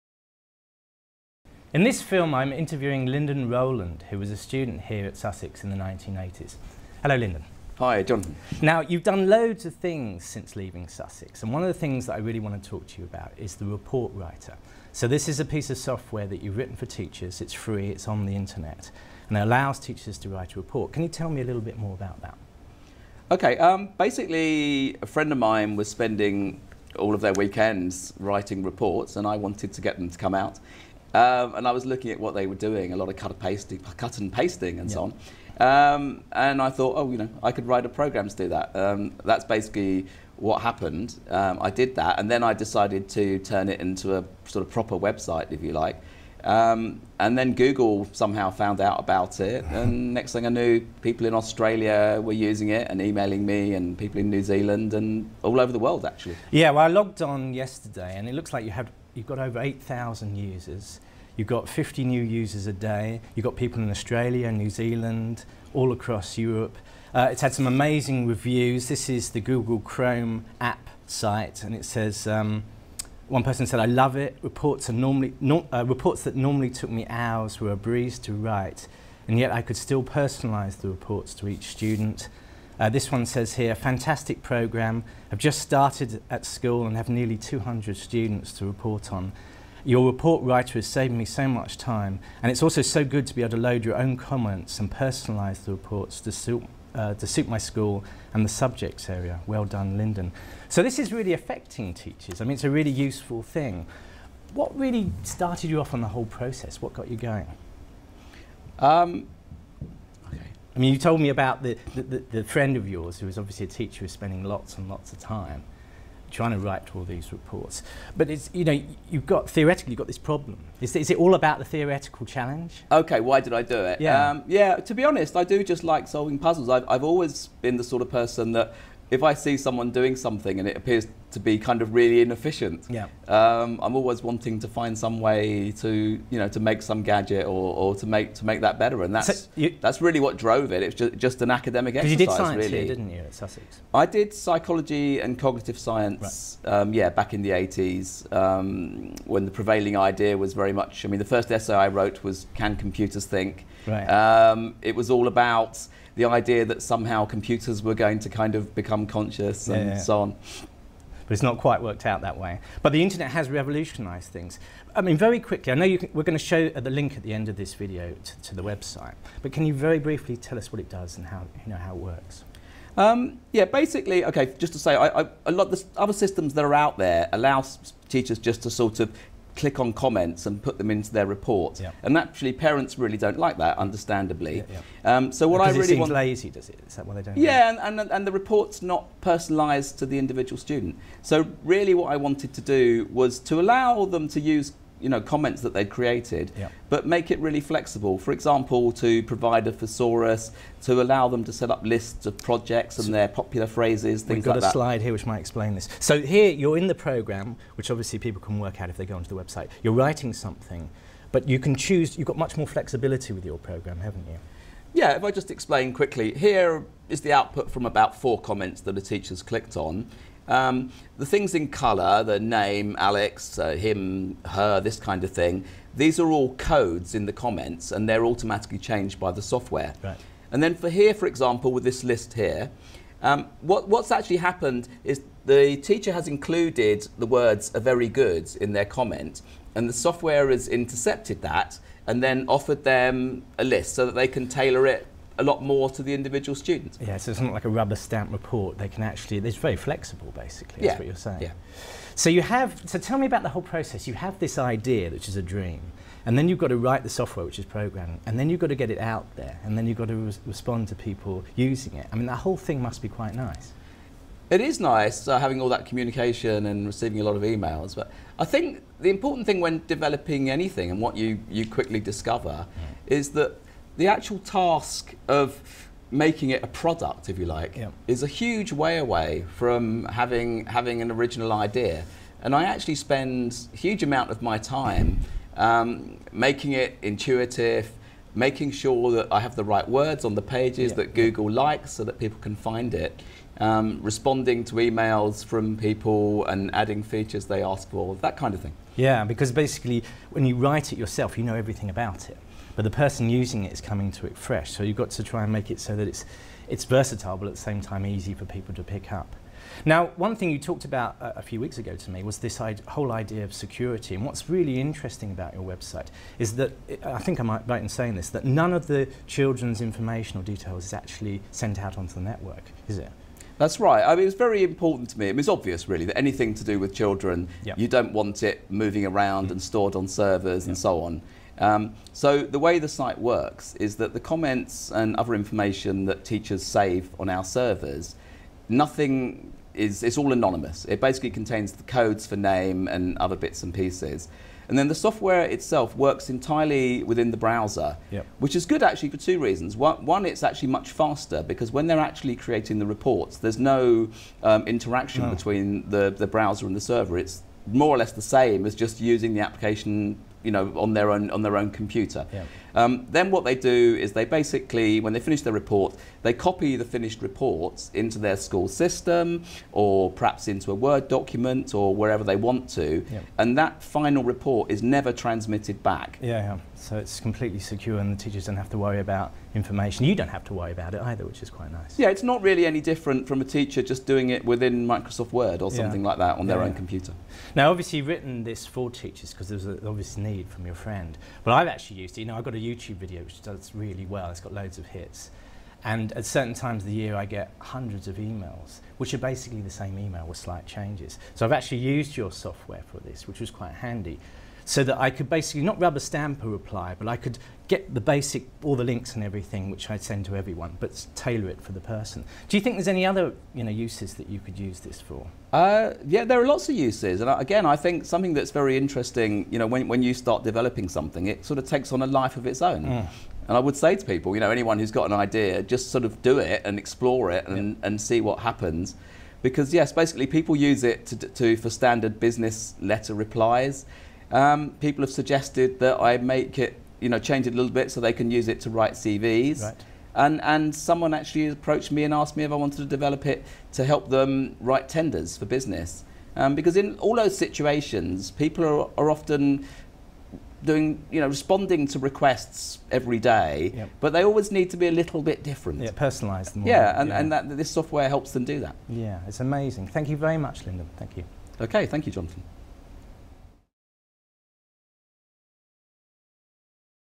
Life in Science - Linden Rowland (developer SchoolReportWriter.COM)
Linden will be talking about a free on-line app which he has developed to help teachers prepare reports. We look at how the idea developed into a prototype and then into a worldwide service. Filmed in 2013.